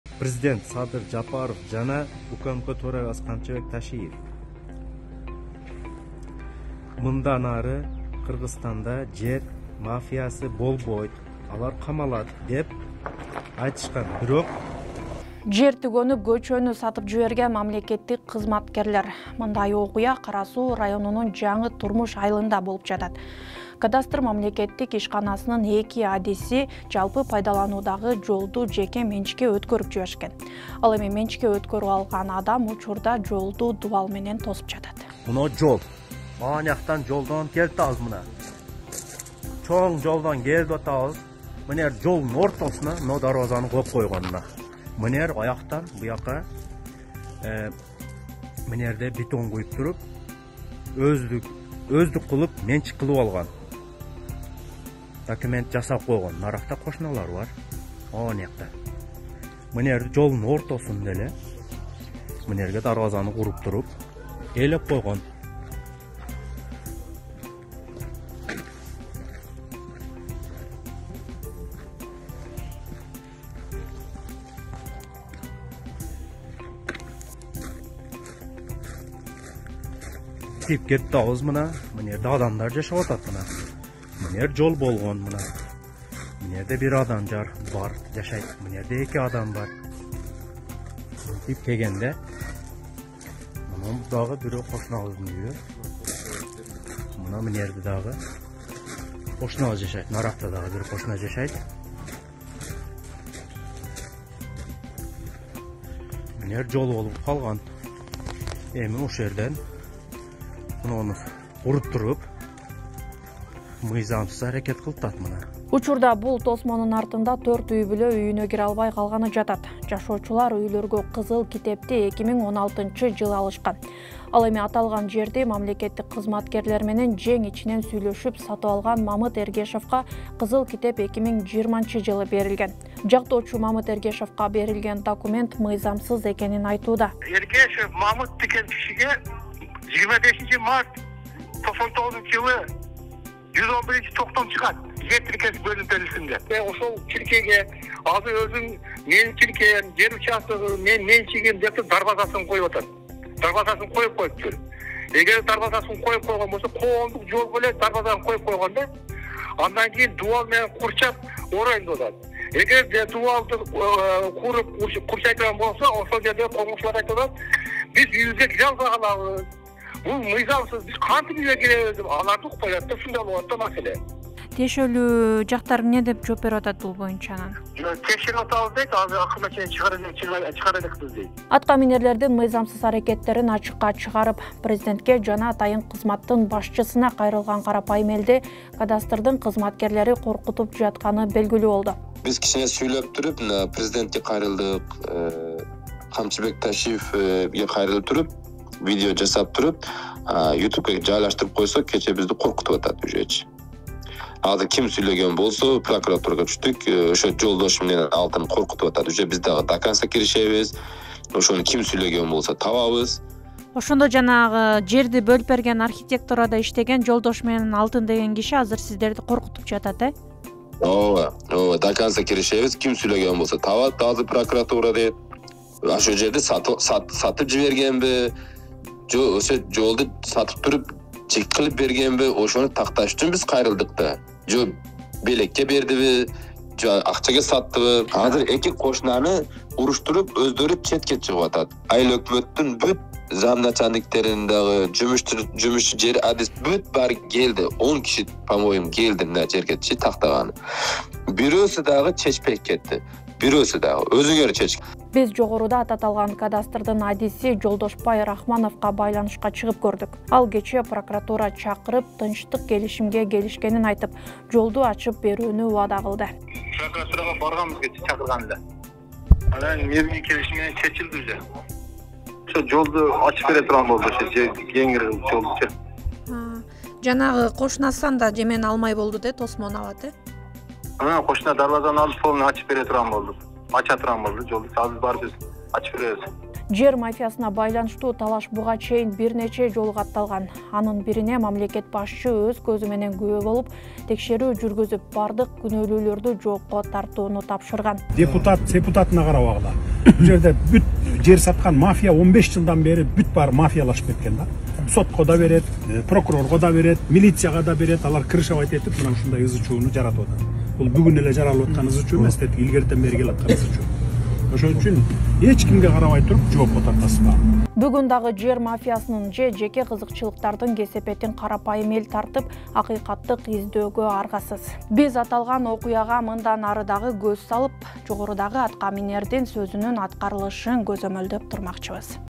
Жерди эмес, КӨЧӨНҮ да сатып жиберген АТКАМИНЕРЛЕР//
Жерлердин сатылып кеткени же “жер мафиясы” дегенди угуп эле жүрсөңүз керек. Бирок бул жолу сата турган жер түгөнүп көчөнү сатып жиберген мамлекеттик кызматкерлер катышкан окуяга туш болдук. Акыйкат издеген жергиликтүү эл редакциябызга кайрылды
 Окуя Кара-Суунун Жаңы-Максат айылында болгон.